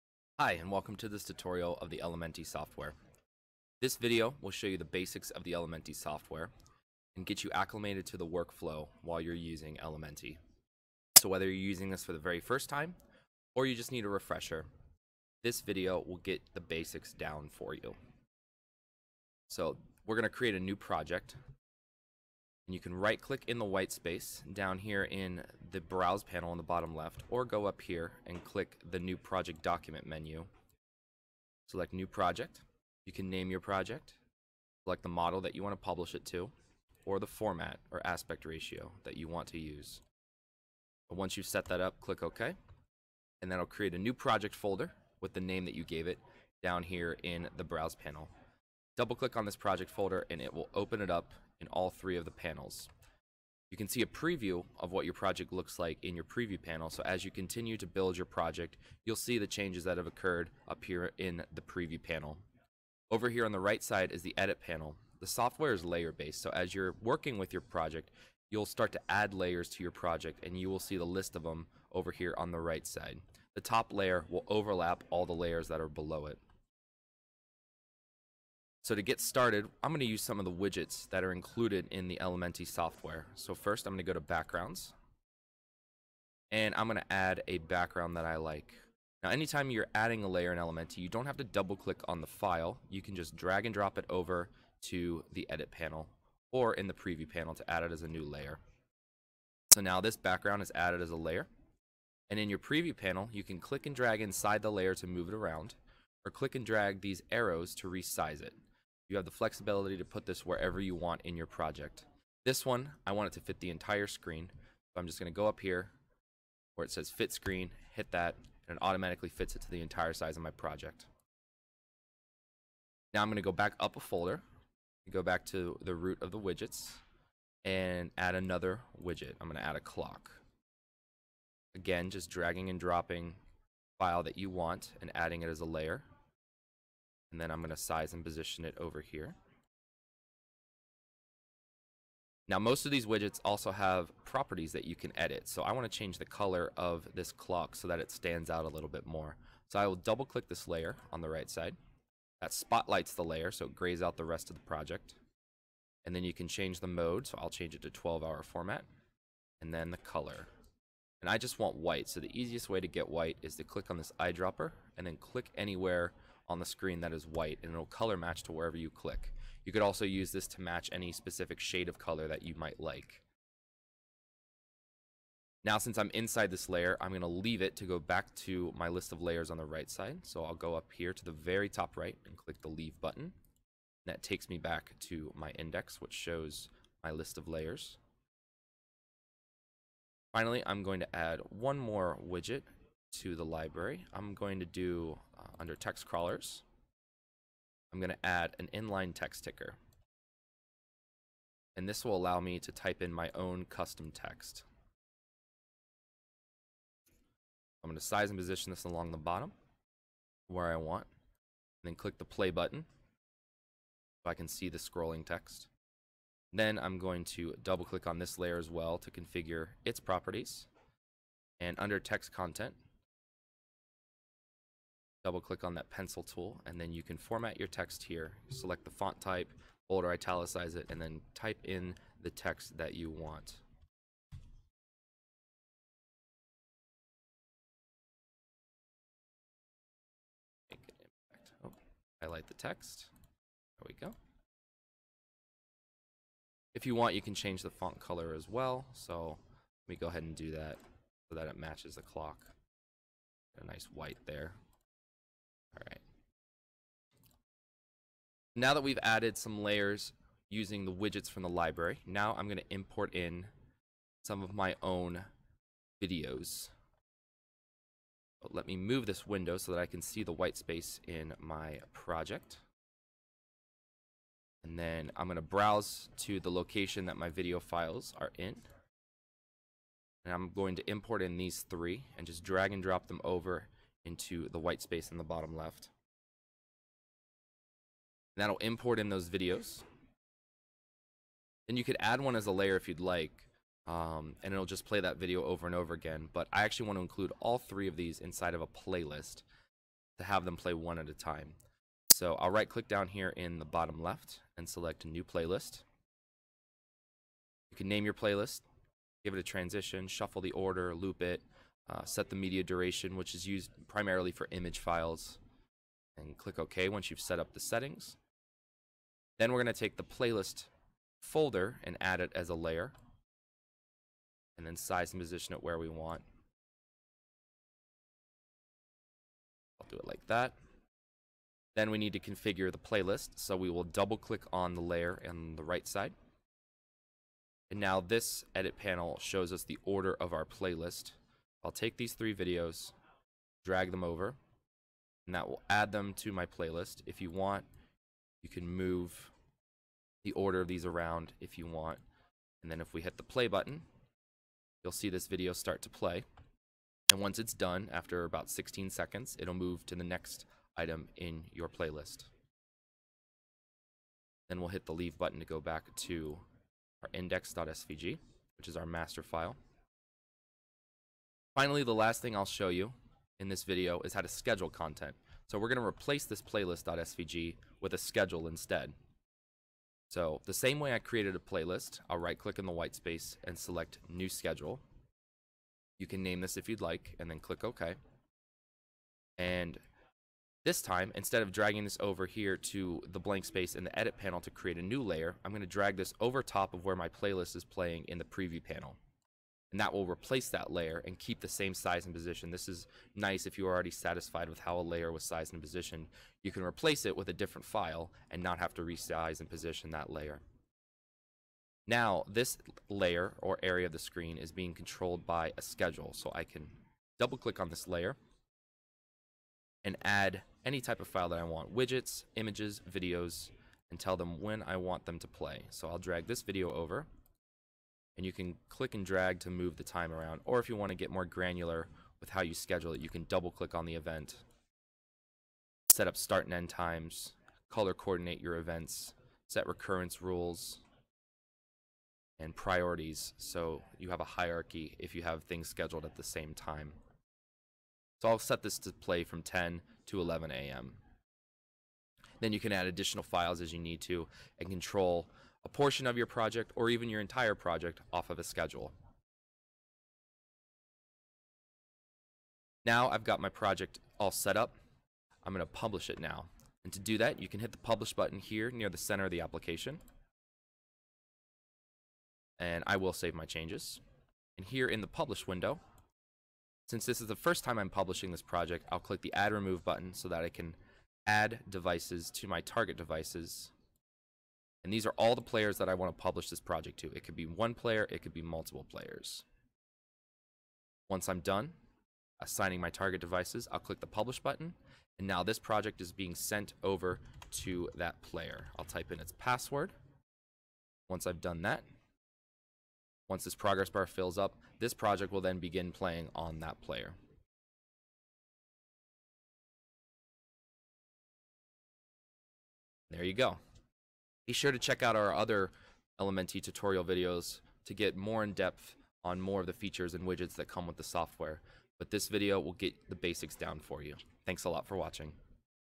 Elementi in Less Than 10 Minutes
Elementi is a powerful and easy to use digital signage software platform that makes creating, managing and scheduling content a snap.